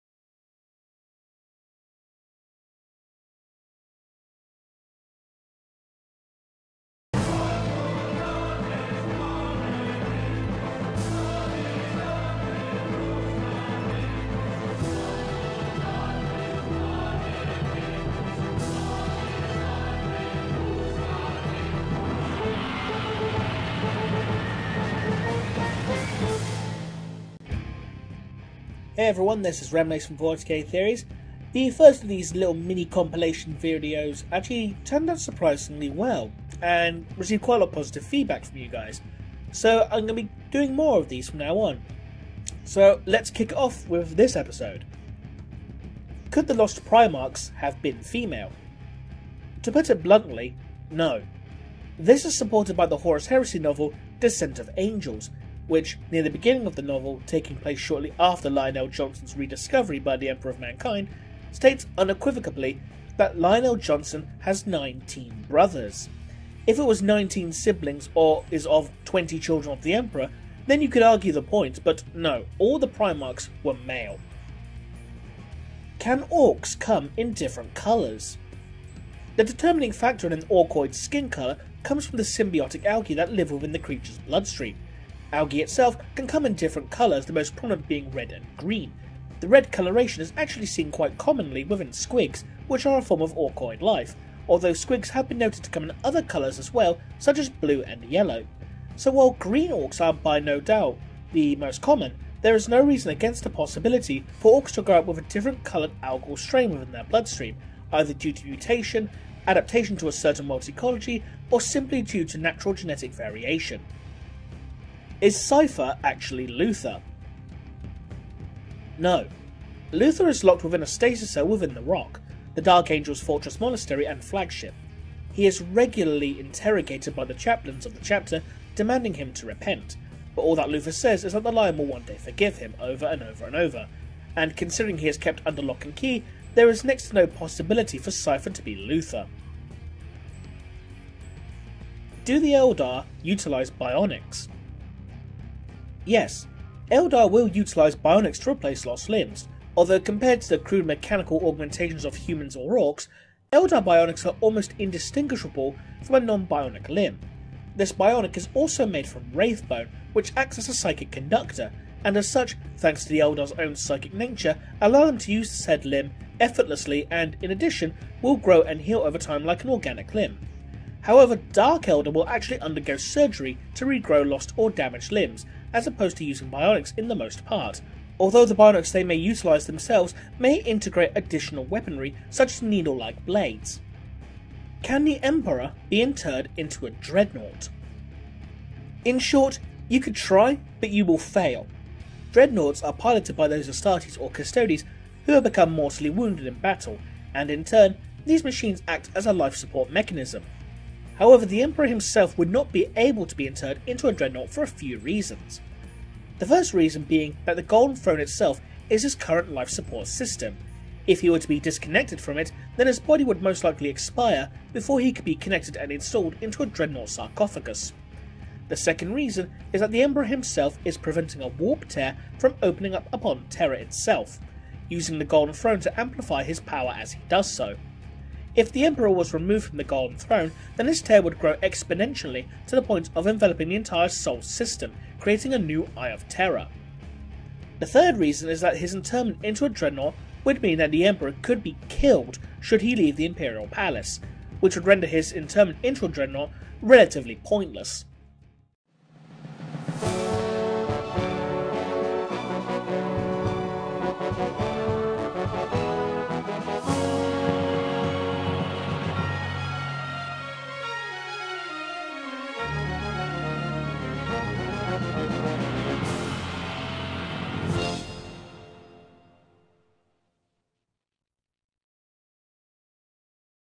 Mini-Theory Compilation 2 - 40K Theories
Remleiz takes a look at a few more questions too short or stupid for a full episode:

*Could the Lost Primarchs have been female?

*Can Orks come in different colours?

*Is Cypher actually Luther?

*Do the Eldar utilise bionics?

*Can the Emperor be interred into a Dreadnaught?